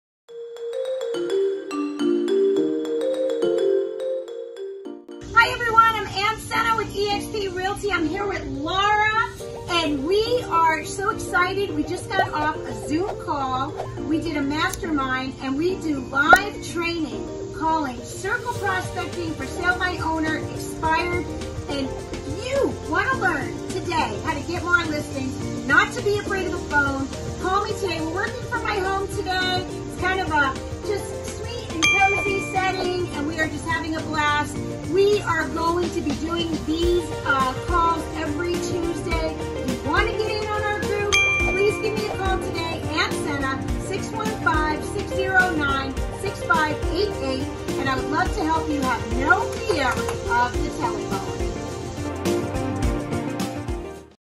EXP Realty | Don't be afraid to pick up the phone
Do you want to learn how to get more listings?  Fear of the phone??  Free coaching and live call session!!